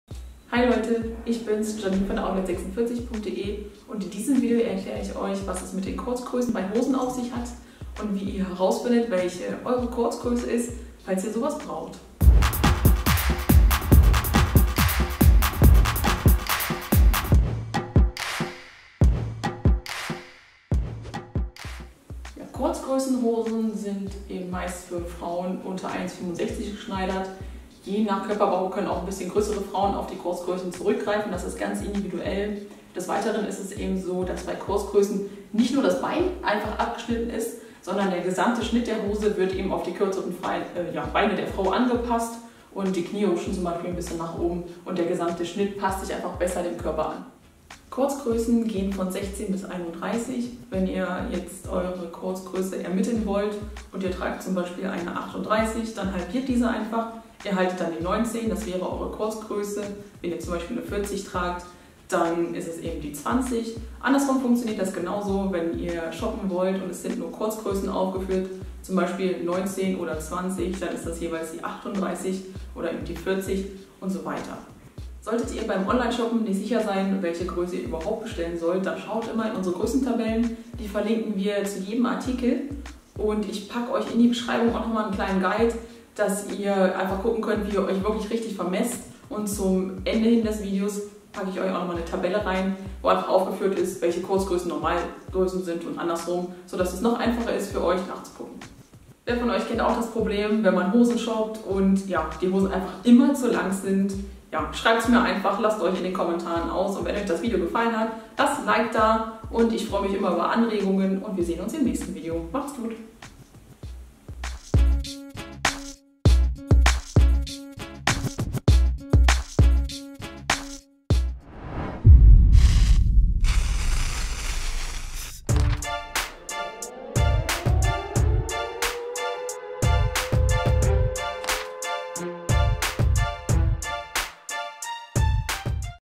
Was sind Kurzgrößen? Jeans Mode in Kurzgröße | Jeans für kleine Frauen | Outlet46 | Tipps & Tricks
Günstig Markenklamotten shoppen!

Kurzgrößen Erklärung by Outlet46

Mit diesen Infos wisst ihr ab sofort genau was Kurzgrößen sind, wie man sie umrechnet & welche Kurzgröße die Richtige für euch ist.

Wir freuen uns über jegliche Kommentare, Feedbacks und konstruktive Kritik zu unseren Videos. :-)